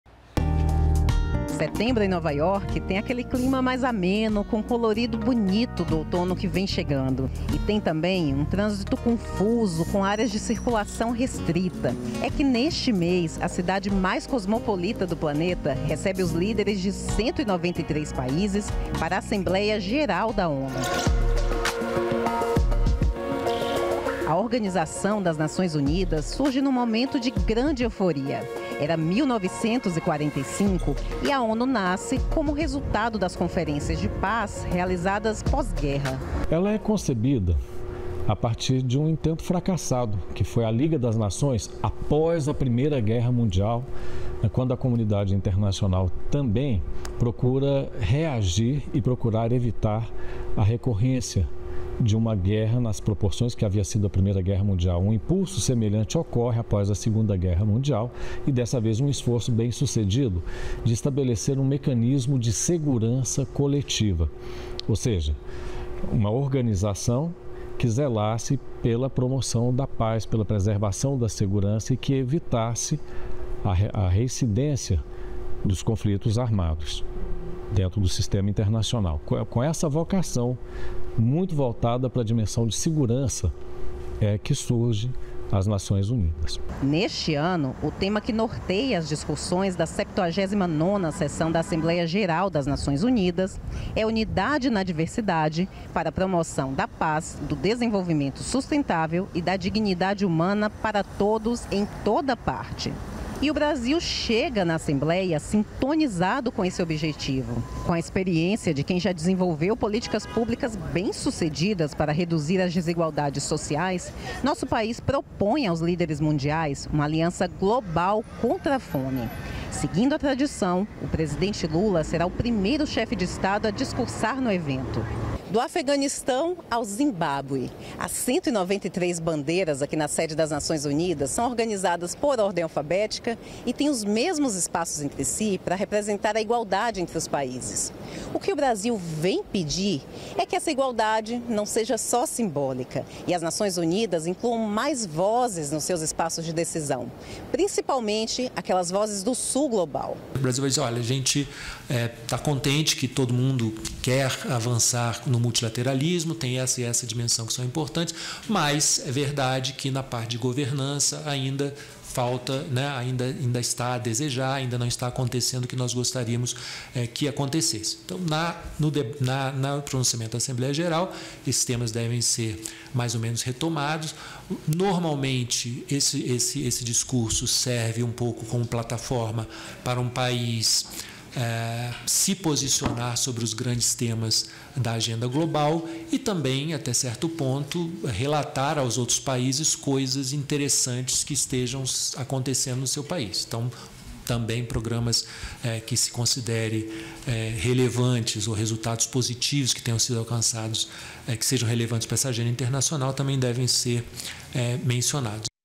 ONU: Lula será o primeiro a discursar na Assembleia Geral em NY
O presidente Luiz Inácio Lula da Silva será o primeiro chefe de Estado a discursar na abertura da 79ª edição da Assembleia Geral das Nações Unidas, que acontece em Nova York na terça-feira (24/09). Desde 1955, o Brasil é o primeiro país a discursar.

O evento com o tema “Unidade na Diversidade para promoção da paz, do desenvolvimento sustentável, e da dignidade humana para todos em toda parte" receberá os líderes de 193 países.